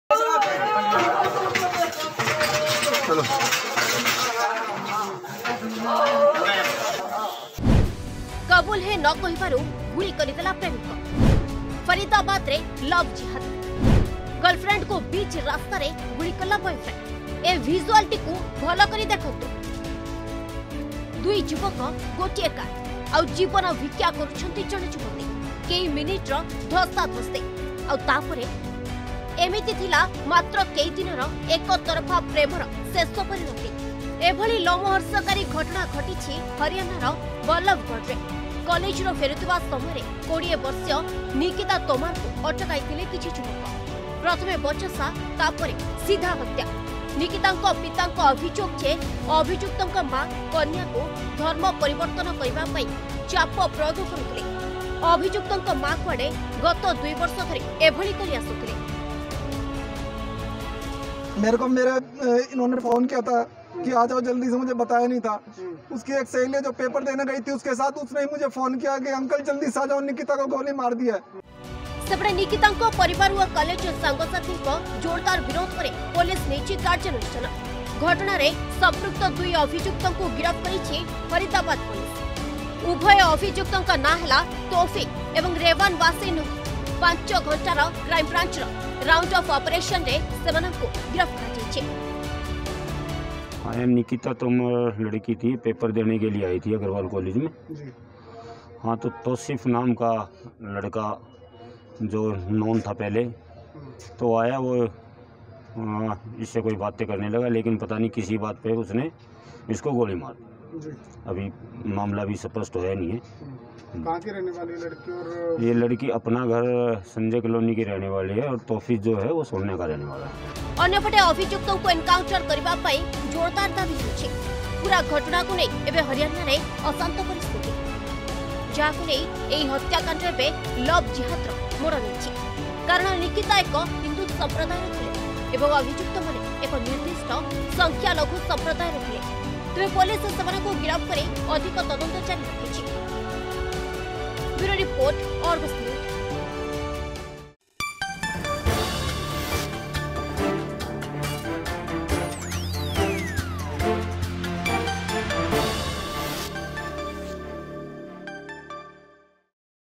‘କବୁଲ ହୈ’ ନ କହିବାରୁ ଗୁଳି କଲା ପ୍ରେମିକ
ଗର୍ଲଫ୍ରେଣ୍ଡକୁ ବିଚ୍ ରାସ୍ତାରେ ଗୁଳି କଲା ବୟଫ୍ରେଣ୍ଡ । ହରିୟାଣା ବଲ୍ଲଭଗଡ଼ରେ ସିସିଟିଭିରେ କଏଦ ହେଲା ଲାଇଭ ମର୍ଡର ଦୃଶ୍ୟ । ନିକିତାଙ୍କୁ ନ୍ୟାୟ ପାଇଁ ହରିୟାଣାରେ ଜୋରଦାର ବିରୋଧ.